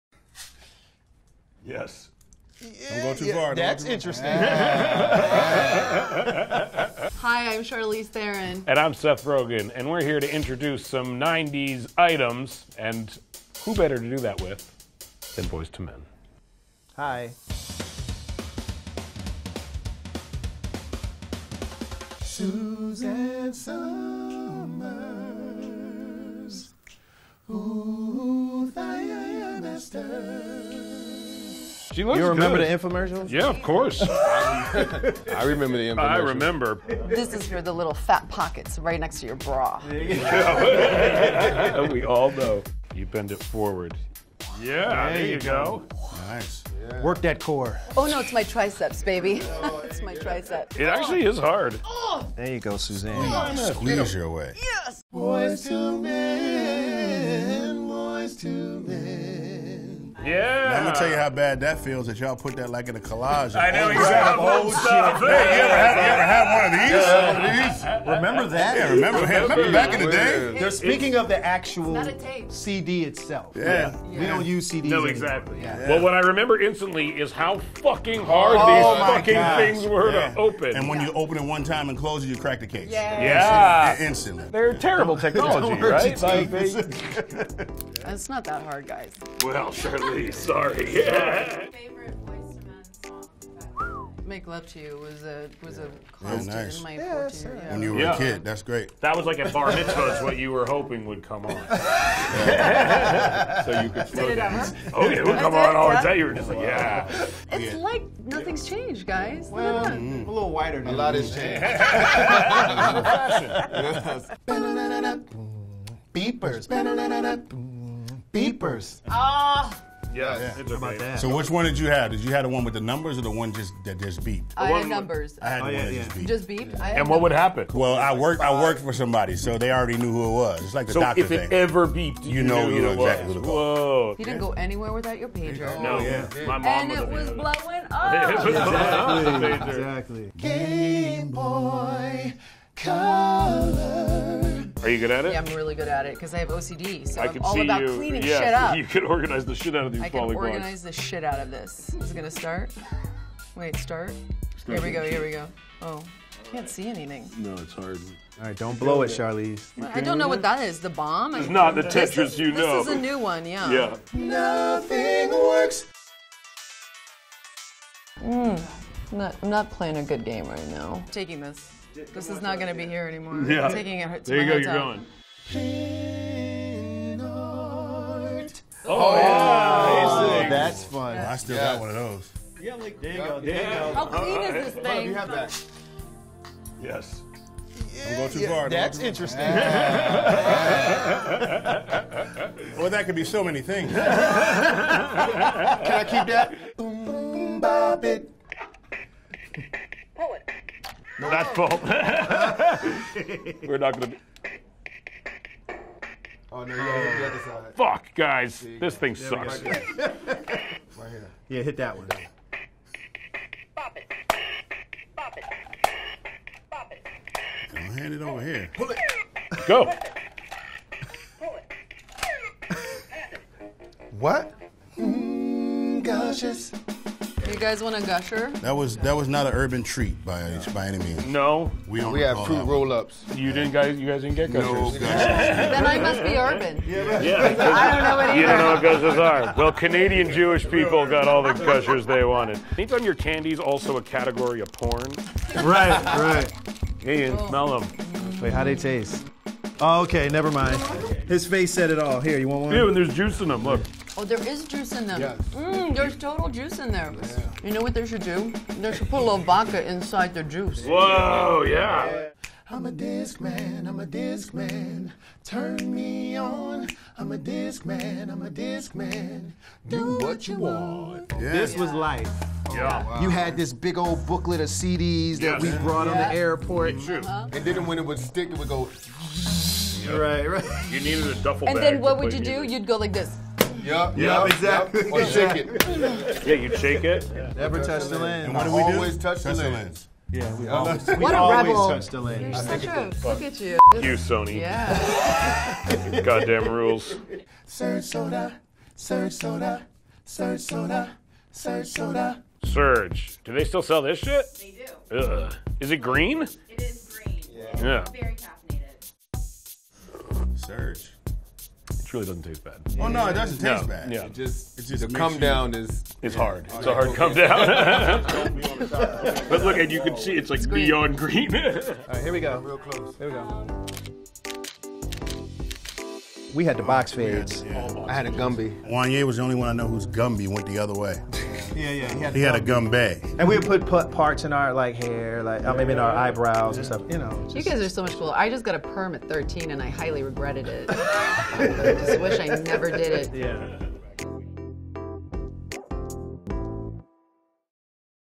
Charlize Theron, Seth Rogen, and Boyz II Men Review the 1990s | Glamour
Charlize Theron and Seth Rogen review 1990s items with the help of Boys II Men, including the ThighMaster, the Game Boy Color, Pin Art, Bop It, and more. Charlize and Seth star in "Long Shot," out in theaters May 3rd.